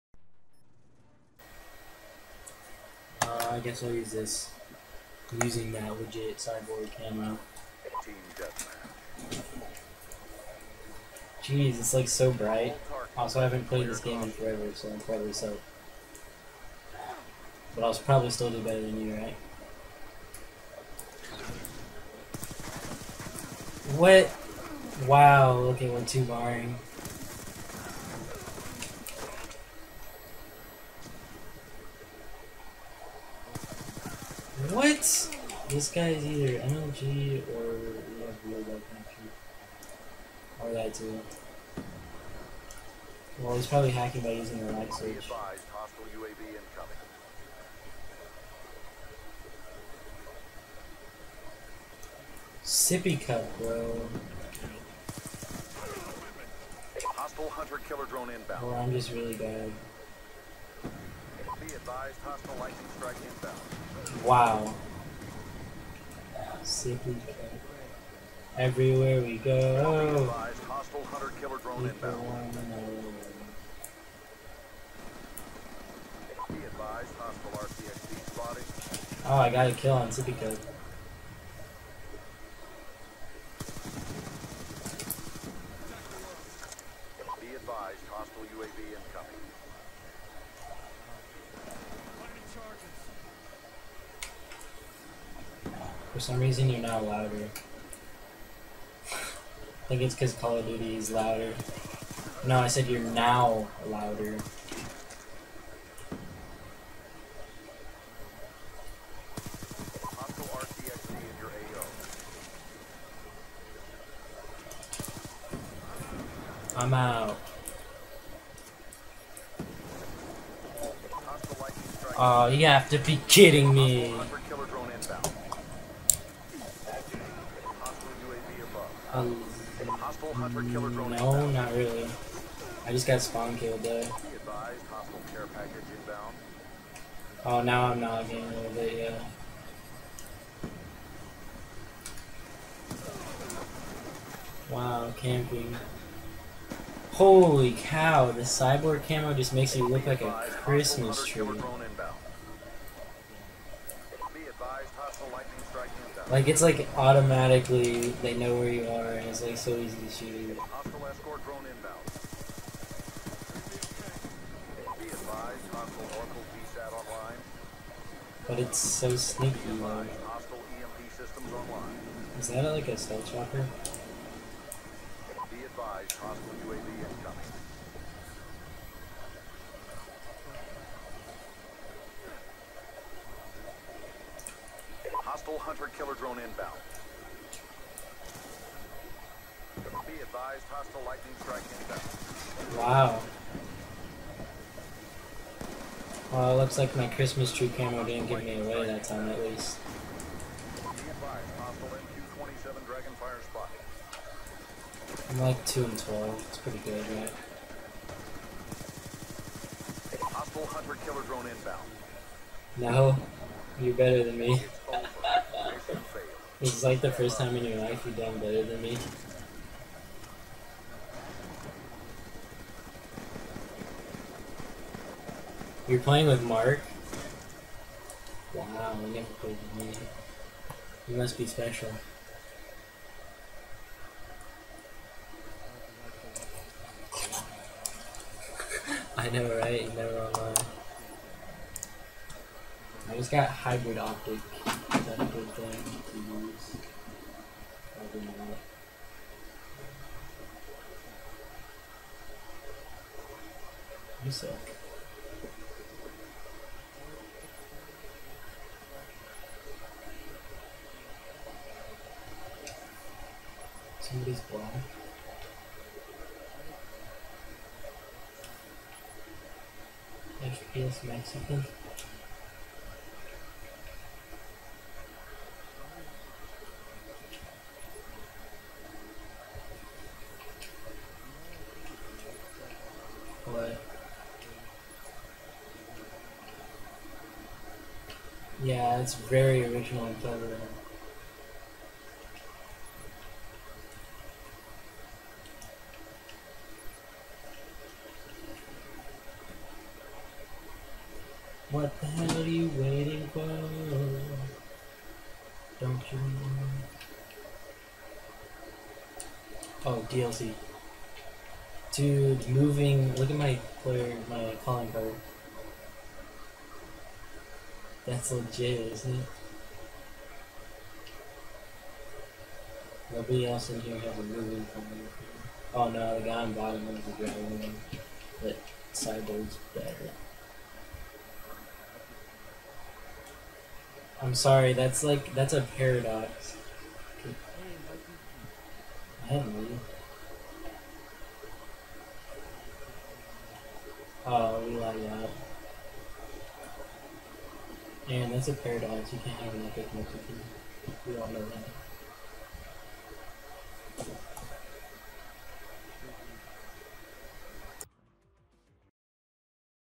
Aeron and I Are Destroyed
Aeron and I were playing on Studio again. We get wrecked pretty hard. Give me a break though, I haven't played in a while. Also, dat Cyborg camo tho.